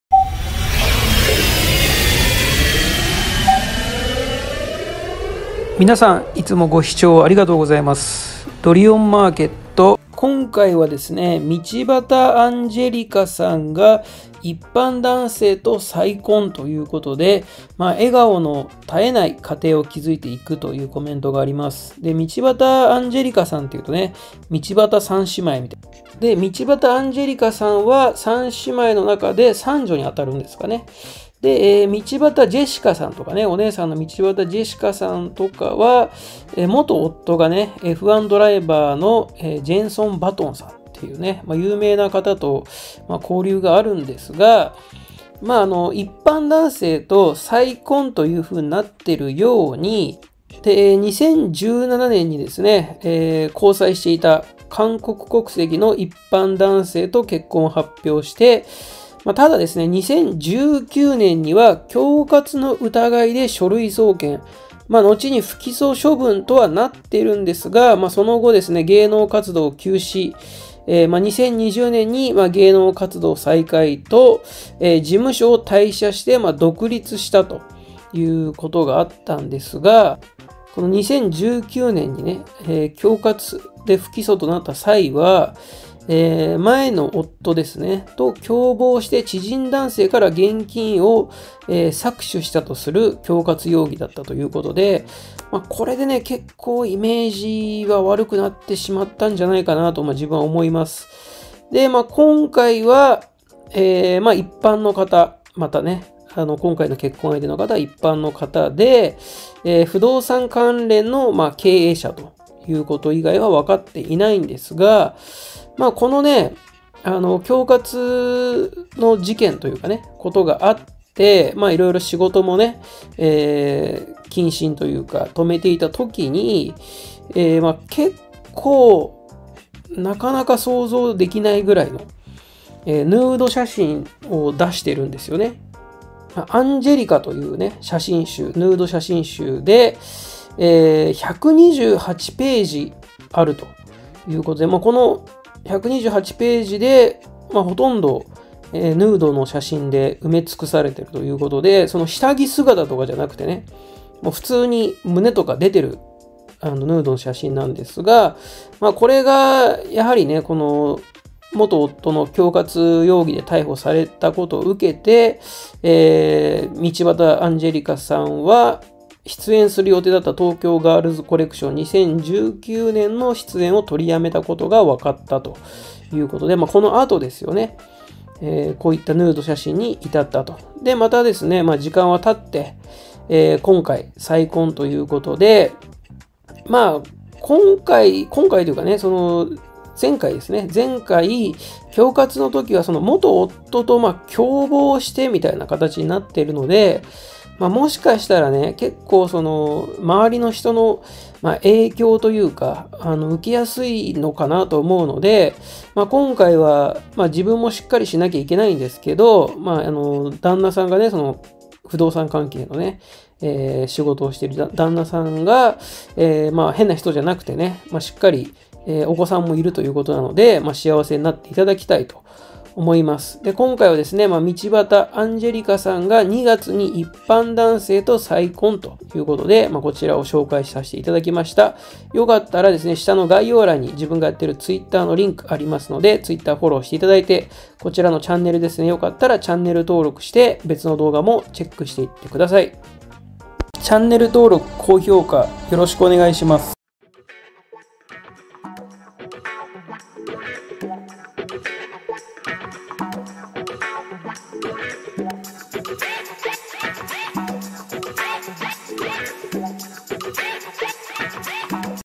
道端アンジェリカ２月に一般男性と再婚！！【恐喝で書類送検→ヌード写真集販売】【芸能人濡れ場【
２０２１年１１月に離婚を発表したモデル、道端アンジェリカ（３７）が２月６日に一般男性と再婚していたことが４日、分かった。